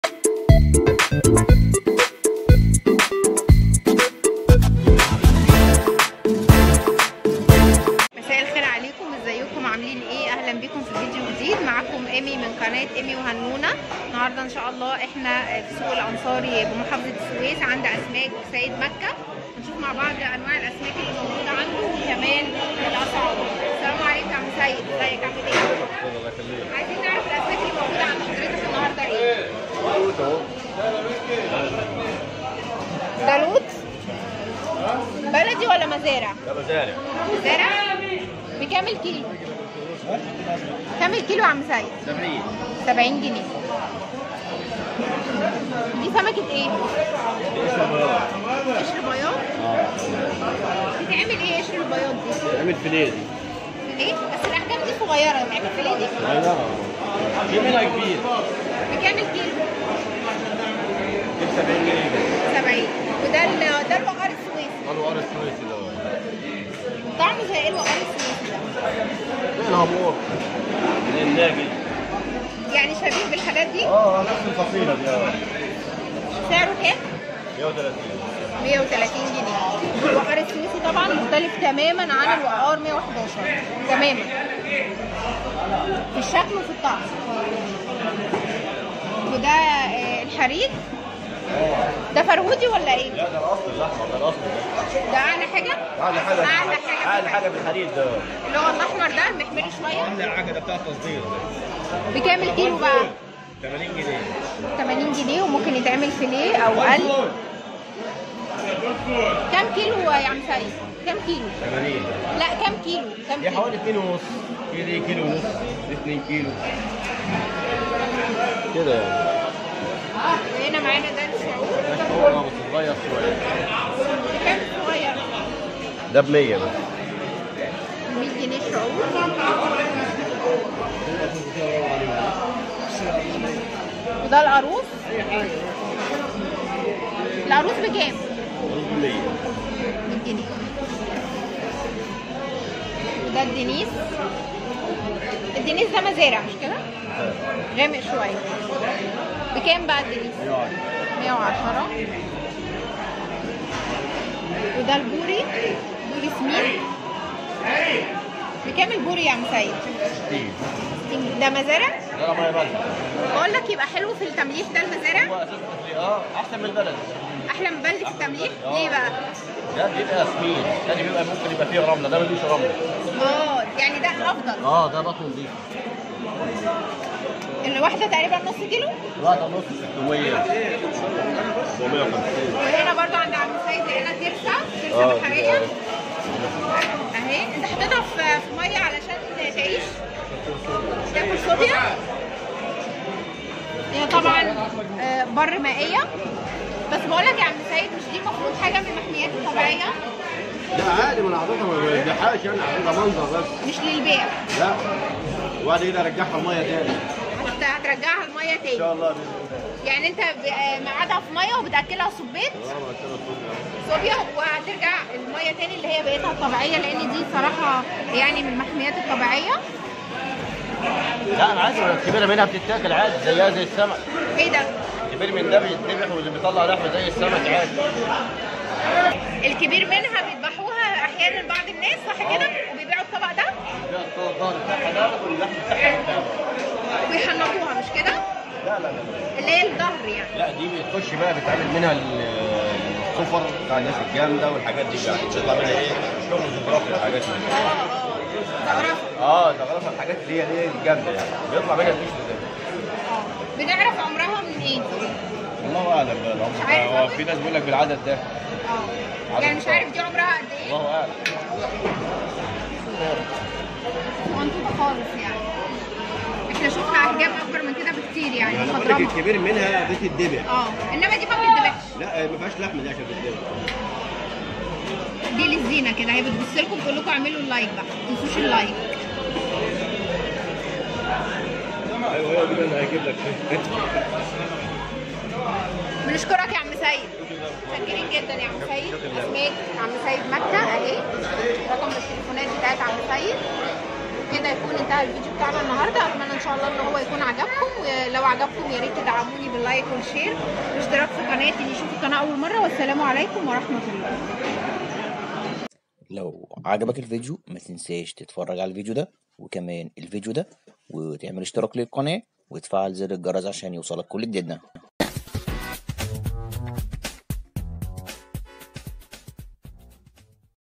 لاقيت سلحفاه بحرية نادره علي فرش لبيع الاسماك بالسويس مع
ظهور السلحفاه البحرية على فرش لبيع الاسماك بالسويس
اصغر سلحفاه بحرية ( ترسه ) معرضة ومهددة بالانقراض
حكايات ( اكسير بحري ) للاسكندرية لشرب دماء وأكل لحم الترسة البحرية
الاسماك المتاحة
اللوت
الشعور
القاروص
قشر البياض
البوري
الدنيس
الترسة البحرية ليست للبيع
السلحفاة البحرية
الحريد الاحمر الاصلي البغبغان
قناة ايمي وهنونة
ملكة البحريات
رقم الوتساب الخاص بالقناة
سوق الانصاري بالسويس اسماك سيد مكة
جولة بسوق الانصاري بالسويس عند اسماك سيد مكة
لعرض الانواع والاسعار للاسماك المتاحة وطرق الطهي المختلفة